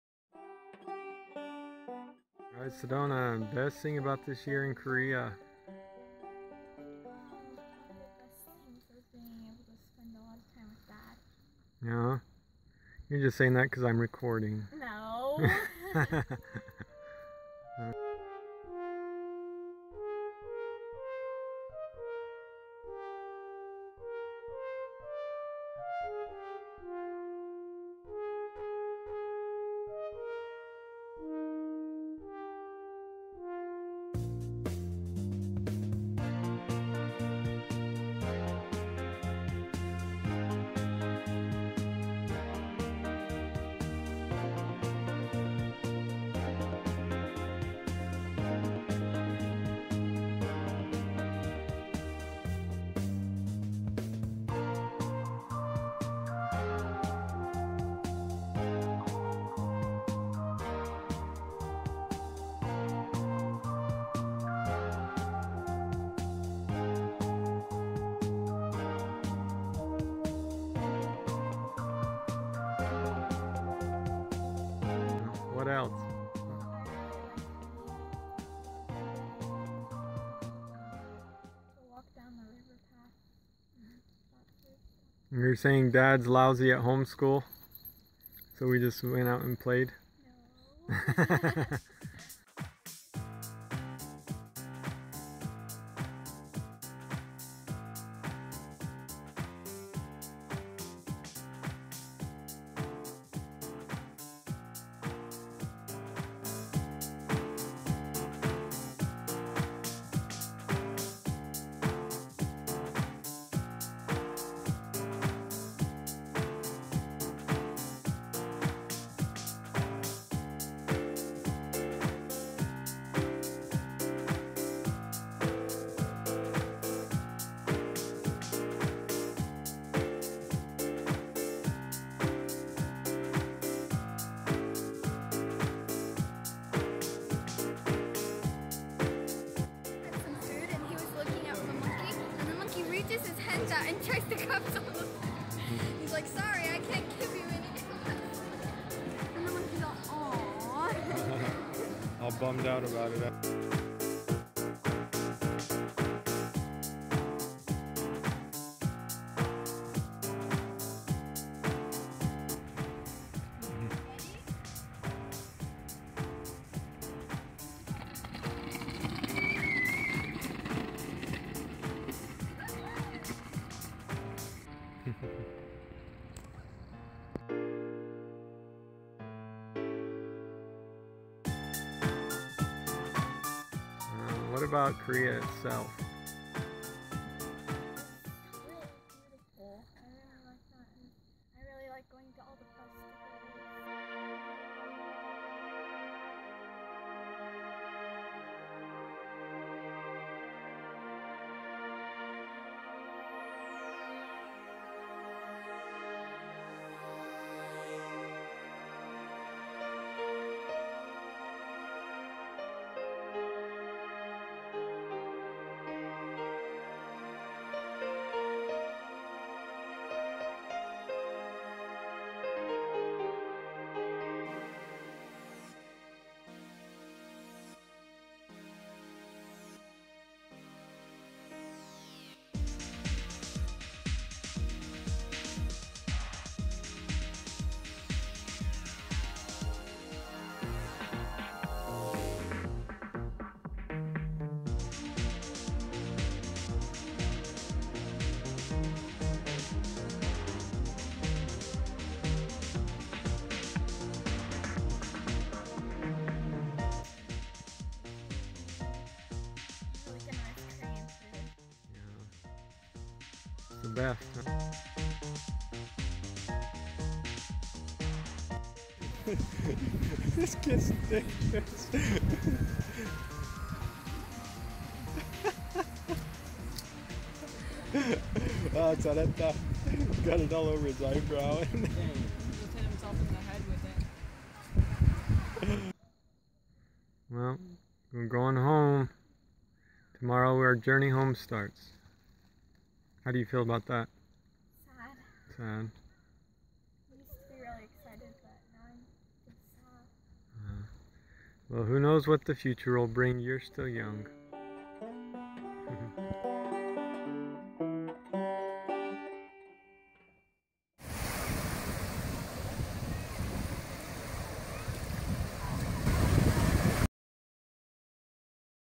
Final Thoughts On Our Year In Korea " Sedona"
The things that Sedona liked most about Korea